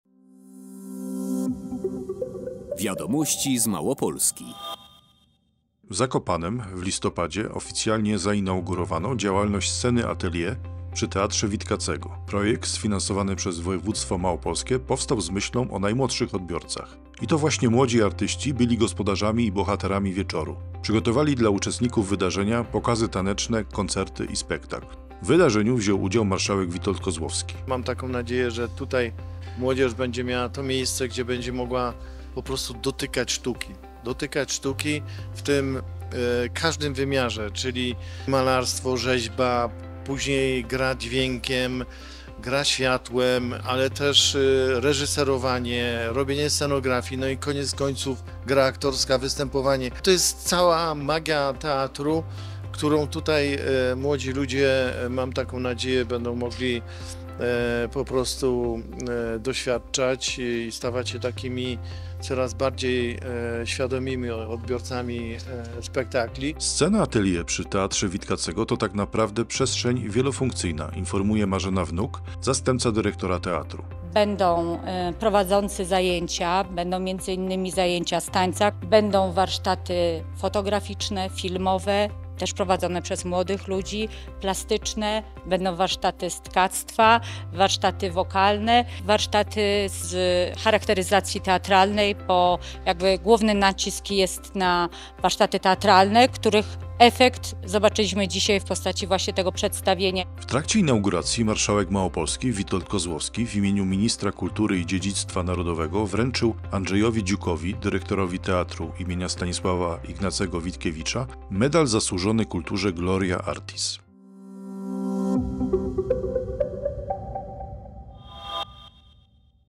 Zainaugurowano działalność Sceny Atelier przy Teatrze Witkacego
W Zakopanem w listopadzie oficjalnie zainaugurowano działalność Sceny Atelier przy Teatrze Witkacego.

Projekt sfinansowany przez Województwo Małopolskie powstał z myślą o najmłodszych odbiorcach.